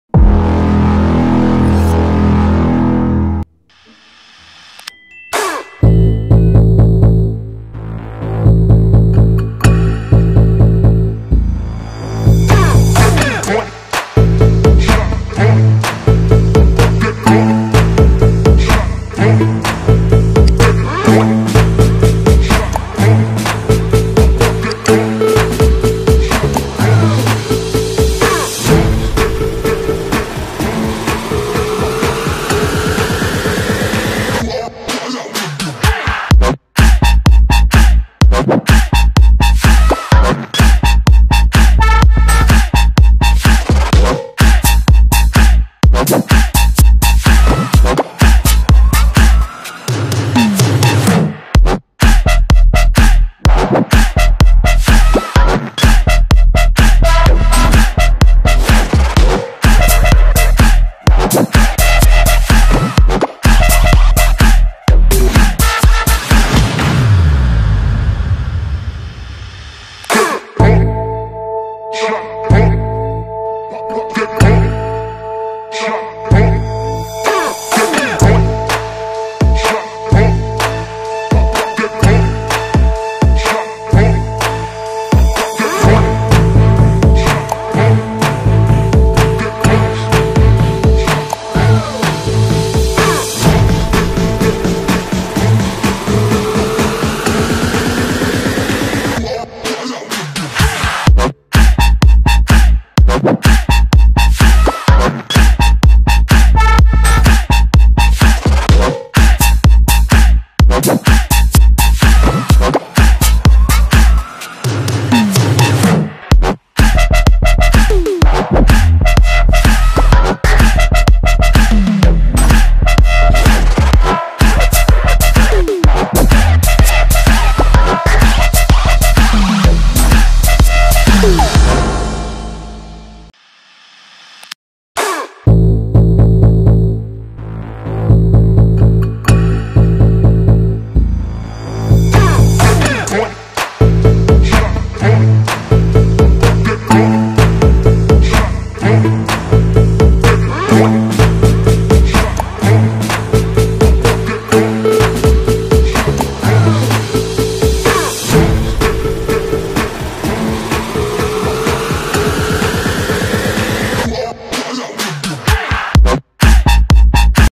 LOS DERRIBOS mas ESPECTACULARES en WRESTLING Olímpico 🔥🔥🔥
Wrestling Motivacional, estos derribos están de locos cual te gusto mas?